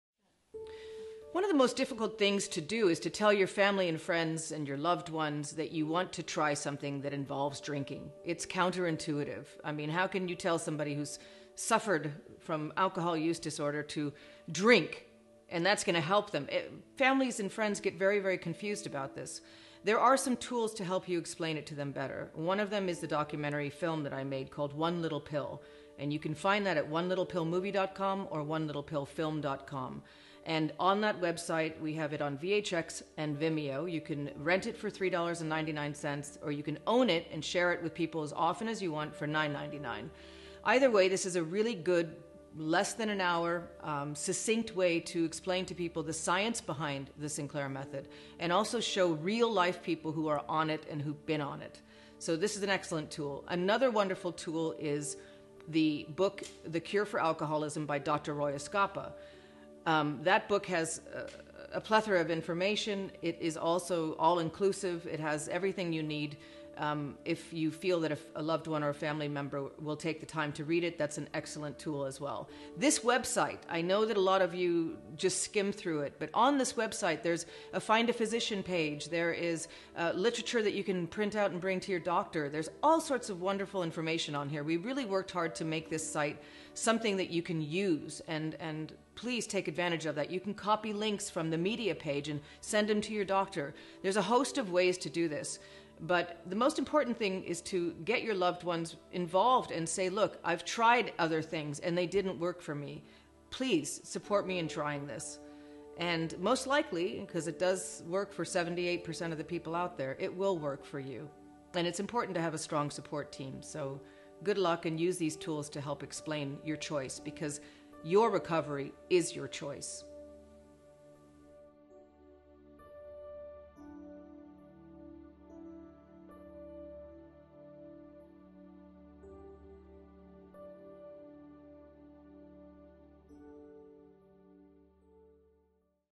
Telling Your Loved Ones About TSM
Your family and friends may be confused when you tell them your treatment involves drinking. Claudia talks about tools that can be used to involve them as supporters in your recovery.